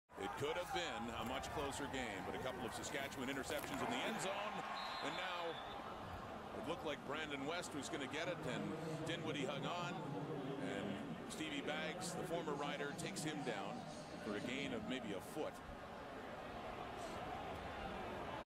Stevie Baggs stuffs Ryan Dinwiddie - October 29, 2011
October 29, 2011 - Stevie Baggs stops Ryan Dinwiddie for a short gain.